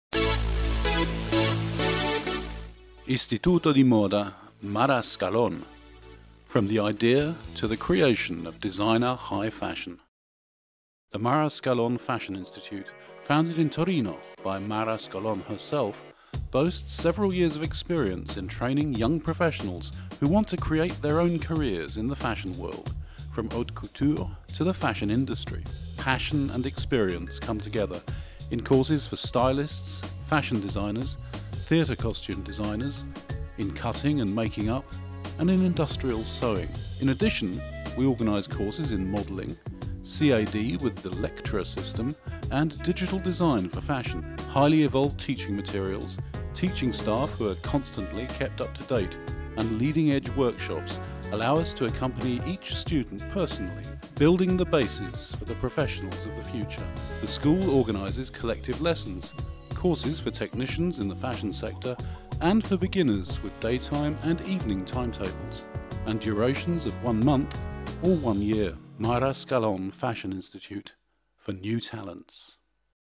Istituto di Moda Mara Scalon TORINO (Torino)
É una scuola di formazione professionale focalizzata sull'orientamento, formazione e addestramento di tecnici della moda. I corsi, tenuti tra tecniche tradizionali e di avanguardia, sono: corsi professionali per modellista, stilista, design della moda, costumista teatrale, tecnico di sartoria, taglio e confezione, maglierista. Dispone inoltre di una sezione di corsi di arte applicata, disegno a mano libera, disegno artistico sulle tecniche di pittura su tela, ceramica, legno, stoffa, vetro. Infine, si tengono corsi di cucito base. Scuola a orario continuato, aperta il sabato e ad agosto. Corsi per professionisti e principianti, diurni o serali, annuali o mensili.